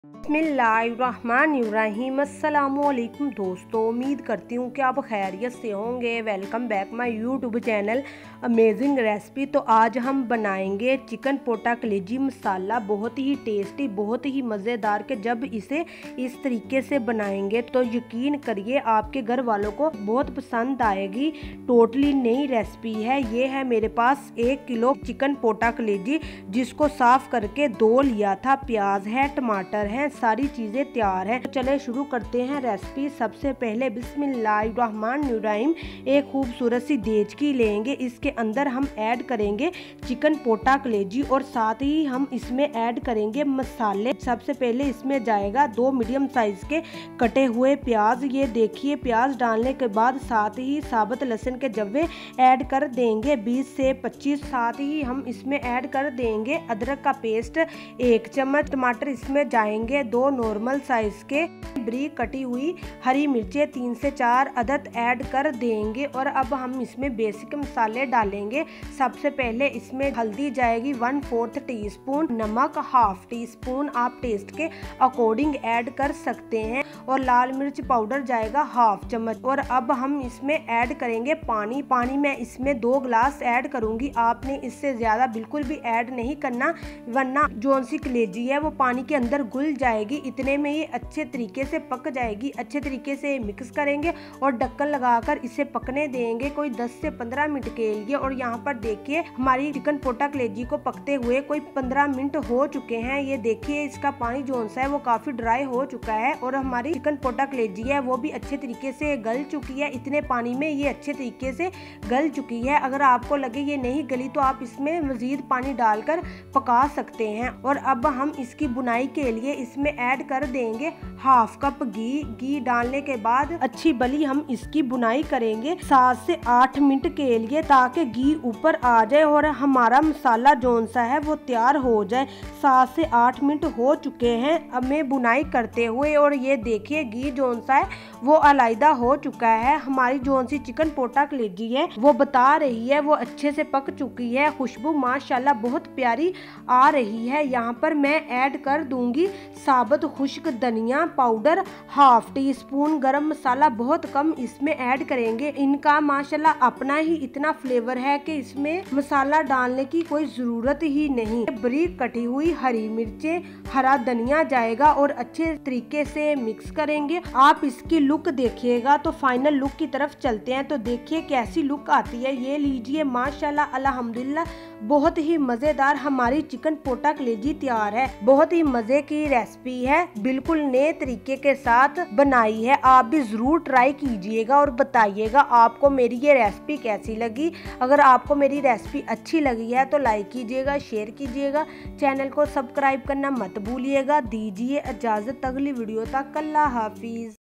Chicken Pota Kaleji Recipe By Amazing Recipe |Pota Kaleji Masala
Watch thHomemadeDelights
About Video: Assalam Alaikum friends, In this YouTube video,
 In this YouTubeis and delicious🍗👩‍🍳 recipe video on "Chicken Pota Kaleji Masala Banane Ka Tarika". Learn step-by-step how to create this savory dish bursting with amazing flavors and aromatic spices. This easy-to-follow recipe will show you the perfect method to cook a flavorful and rich Chicken Pota Kaleji dish that your taste buds will absolutely love. Whether you are a cooking enthusiast or just looking to try something new, this recipe tutorial is perfect for you! So, grab your apron and get ready to impress your friends and family with this delightful homemade meal. Don't miss out on this ultimate recipe guide!